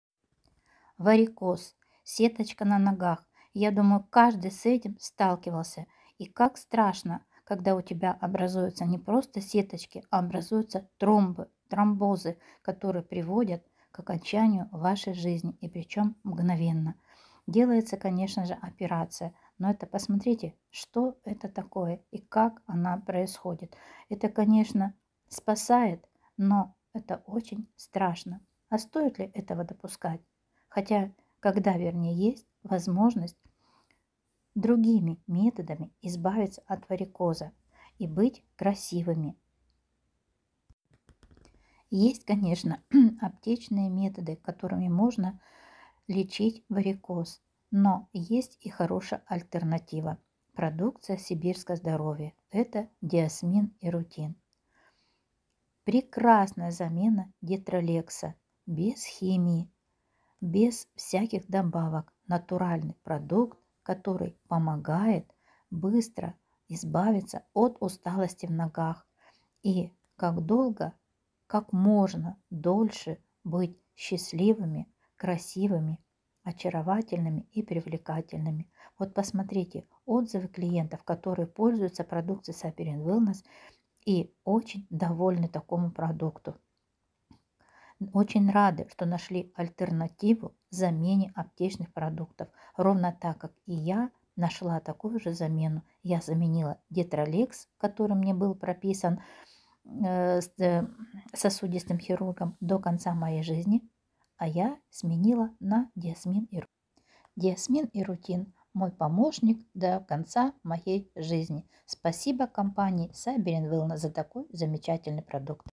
Варикоз|ДИОСМИН И РУТИН замена Детролекса
Диосмин и рутин

Натуральный венотоник на основе диосмина и рутина для предотвращения нарушений лимфотока и кровообращения в ногах. Благодаря оптимальным дозировкам комплекс эффективно воздействует на стенки сосудов, укрепляет их, улучшает лимфоток и микроциркуляцию крови.